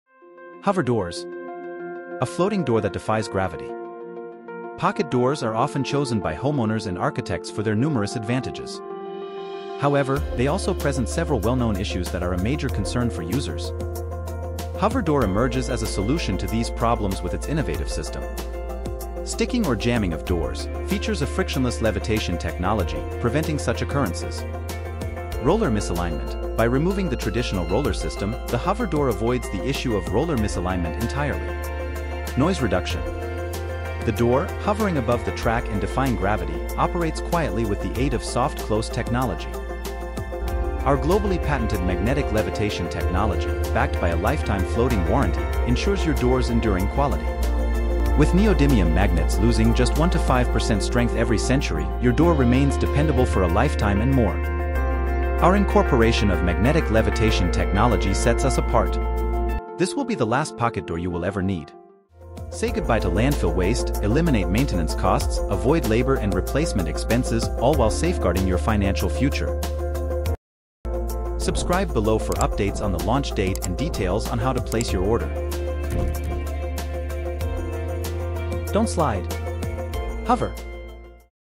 Pocket Door: First-Ever Magnetic Levitation
Elevate your shower experience with Hover Door – the last Pocket Door you'll ever need.

Our globally patented magnetic levitation technology, backed by a Lifetime Floating Warranty, ensures your door's enduring quality. With neodymium magnets losing just 1-5% strength every hundred years, your door remains dependable for a lifetime and more.

Zero Gravity
No Friction
No Support Rollers
No Noise
No Maintenance
No Replacement
Lifetime Warranty